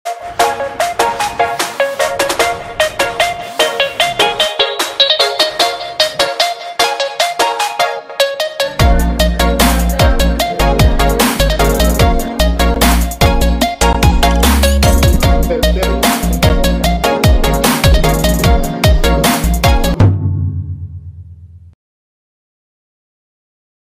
Behind The Scenes; Pottv Team Photoshoot
We at Pottv, did a thing! This is how our team photoshoot went.